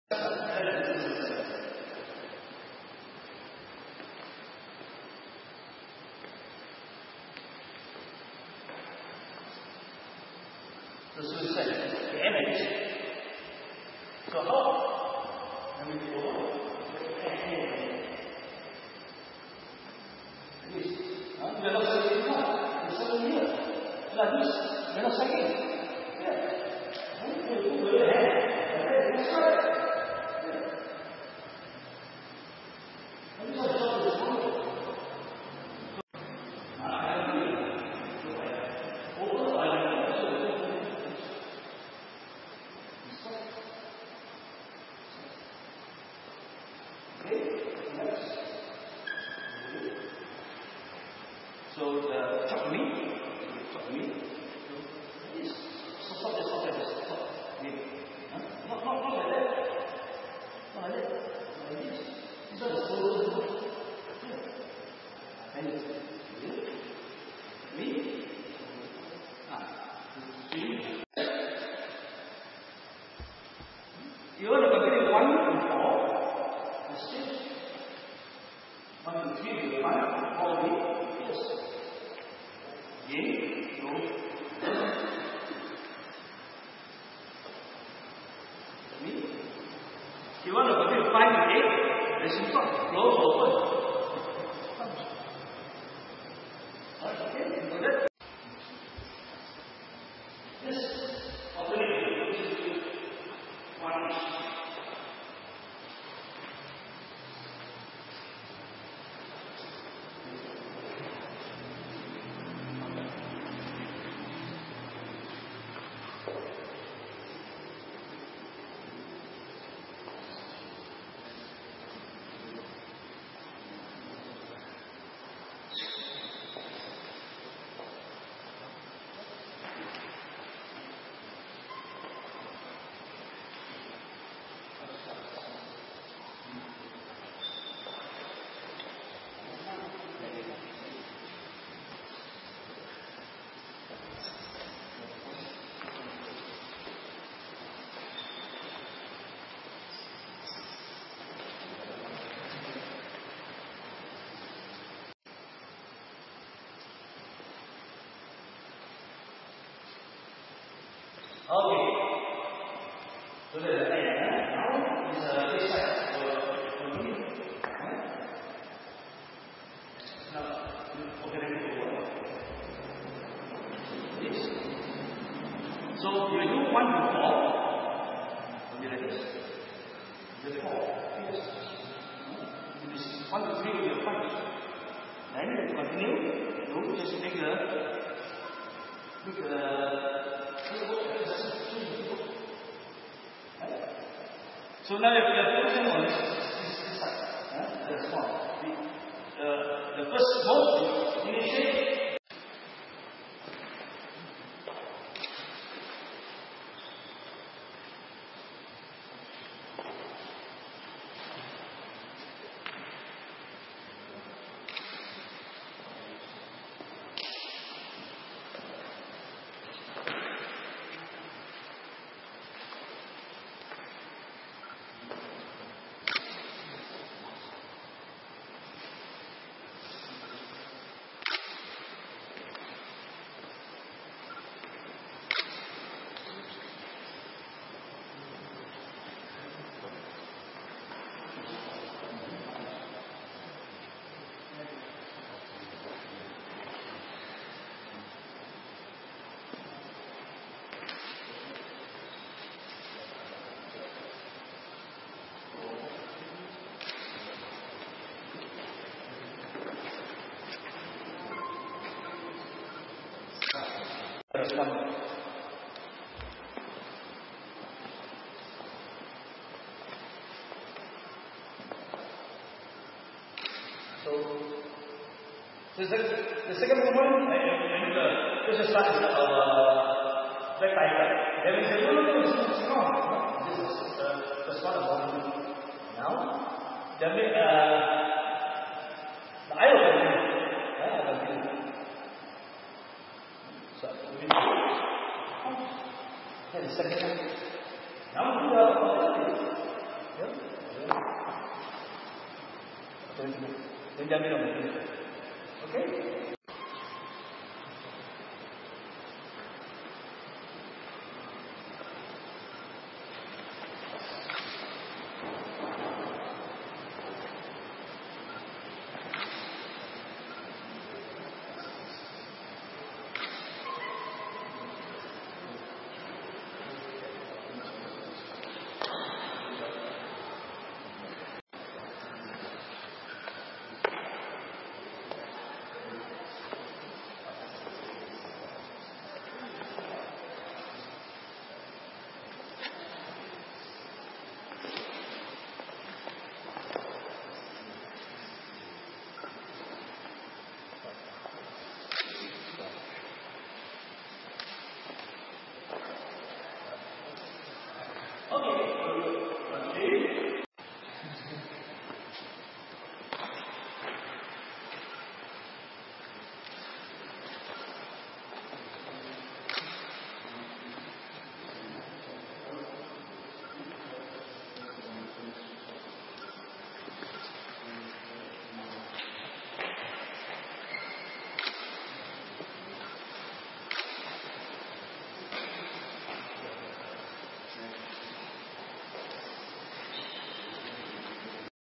Adding and Subtracting Patterns to Confuse Opponents
Intensive Shaolin Kungfu Course in Sabah from 29th June to 5th July 2010.